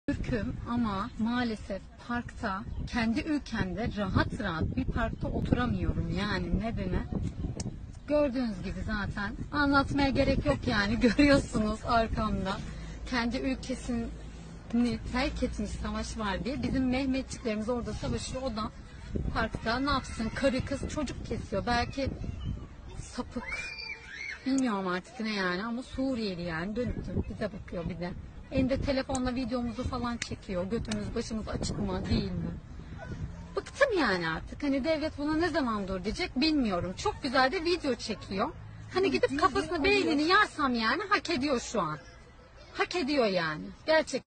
Suriyeli Biri daha kalçamın fotoğrafını çekiyor parklarda oturamıyoruz işte arkadaşlar, görüyorsunuz
“Kendi ülkemde rahat rahat bir parkta oturamıyorum, görüyorsunuz ülkesini terk etmiş bir insan, bizim mehmetçiklerimiz orda savaşıyor o da n’apsın parkta karı, kız, çocuk izliyor. Elinde telefon videomuzu çekiyor. Bıktım yani artık, çok güzel de video çekiyor (!)”